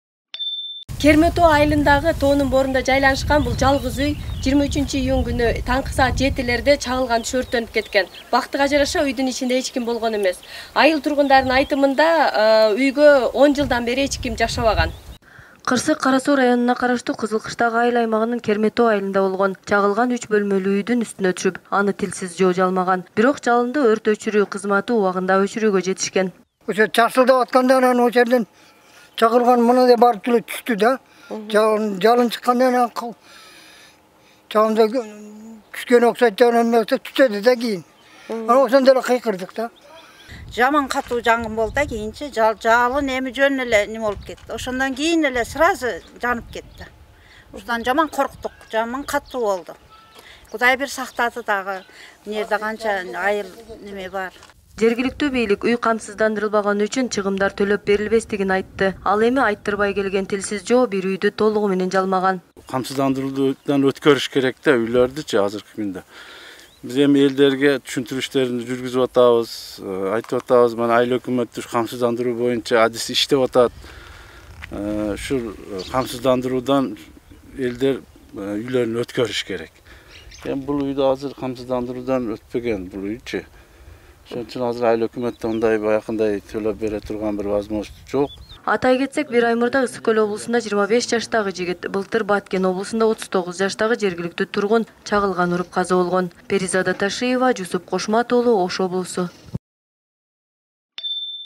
Ош: Чагылгандан бир үй өрттөндү
Ош облусунун Кызыл Кыштак айыл аймагына караштуу Керме Тоо айылында чагылгандан бир үй өрттөндү. Жалпы аянты 72 чарчы метр болгон турак үй толугу менен күйгөн. Өрттүн чыгышына чагылгандын оту себеп болгону айтылды. Бирок кароосуз калган үй болгонуна байланыштуу адамдар жабыркаган эмес.